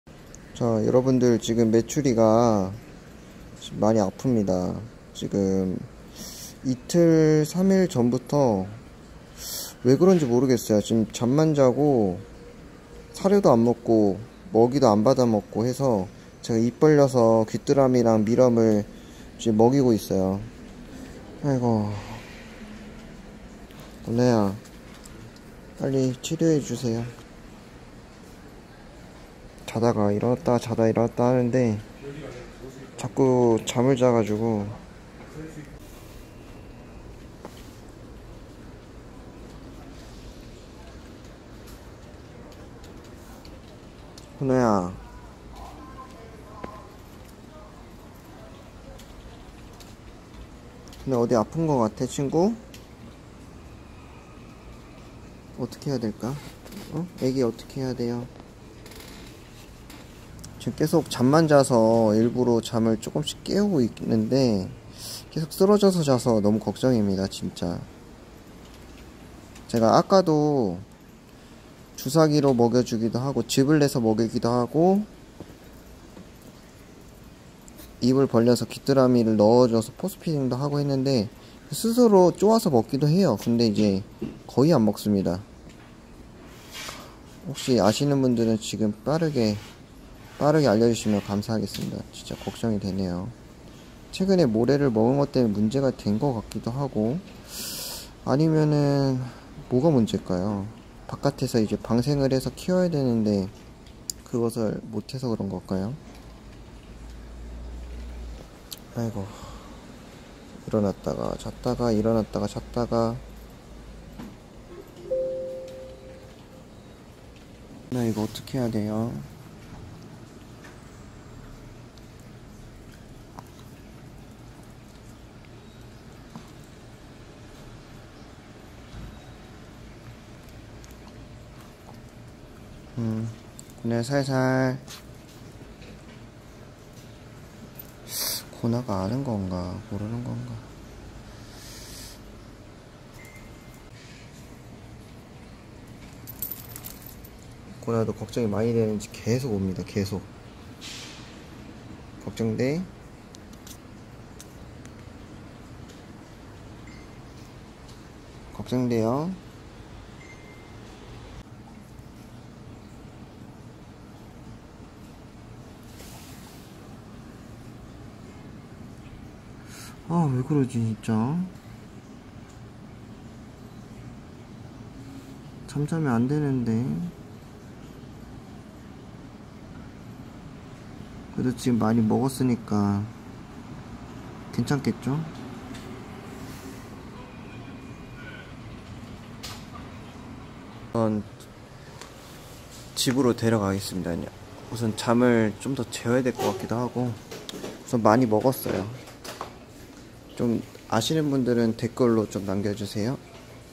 메추리가 죽을것 같아요..도와주세요... [정브르]
먹이도 충분하고 물도.. 은신처도.. 다 좋은데
왜 그럴까요?